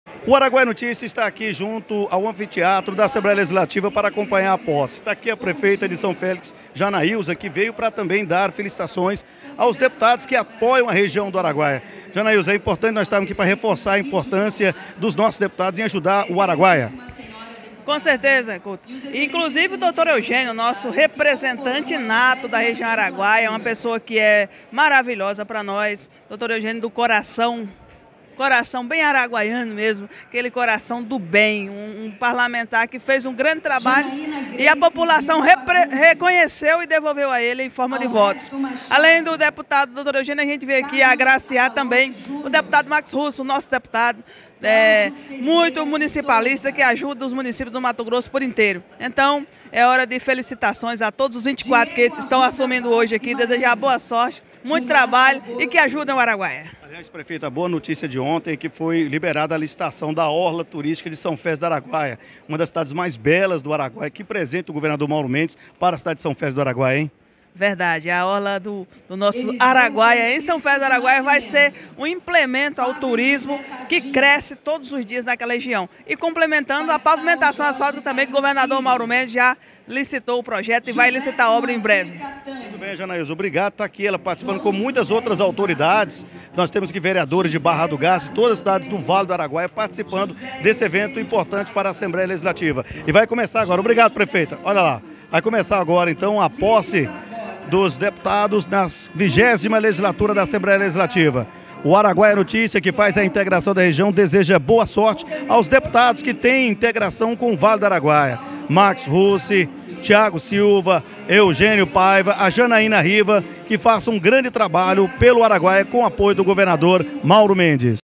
Assembleia de MT empossa deputados para novo mandato
O site Araguaia Notícia está em Cuiabá acompanhando a posse dos deputados estaduais para vigésima legislatura da Assembleia Legislativa de Mato Grosso (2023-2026).